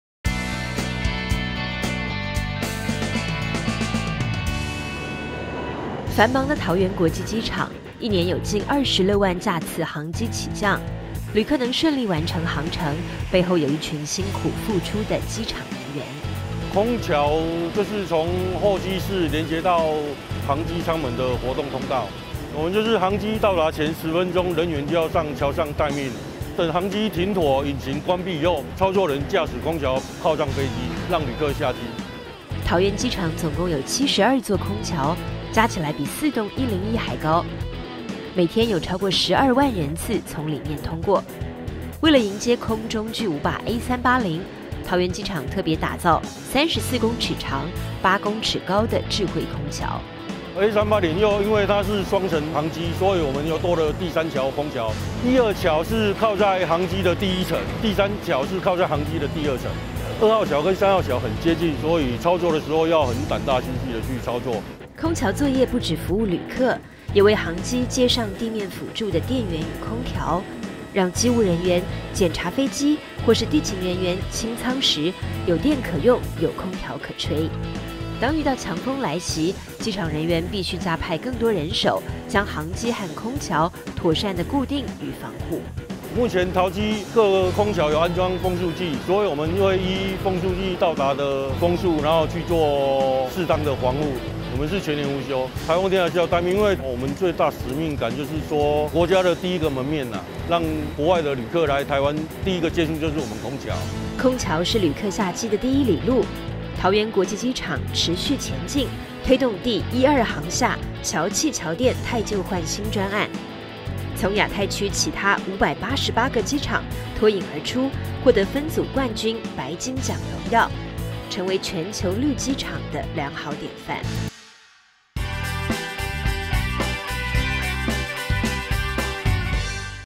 飛行日常-桃園機場百工(空橋篇)
桃園機場每天超過700架次的航班起降✈
空橋是旅客下機的第一哩路， 銜接每趟旅程。

你知道現有的72座空橋加起來比4座101還高嗎?😲
跟著影片一起走進空橋作業人員的機場日常吧！

Taoyuan Airport now handles more than 700  takeoffs and landing every day using air bridges to connect between passengers and each journey.

Do you know the total length of 72 air bridges are 4 times more than the height of Taipei 101?😲

Let’s see what a day in the life of an air bridge operator is like!